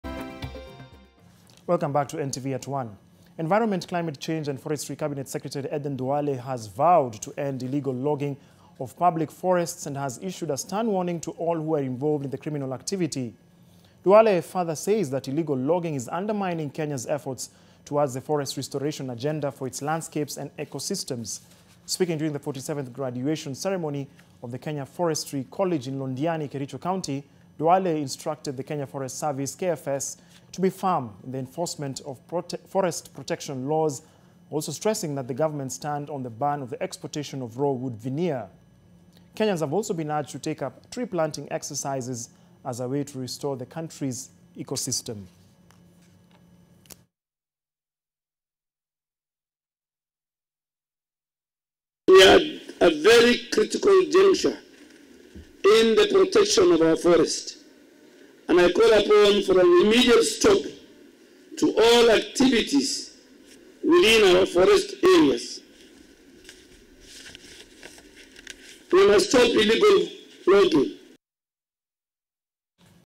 Environment CS Aduale vows to end illegal logging of forests, issuing stern warning to all involved
Environment, Climate Change and Forestry Cabinet Secretary Aden Duale has vowed to end illegal logging of public forests and has issued a stern warning to all who are involved in the criminal activity.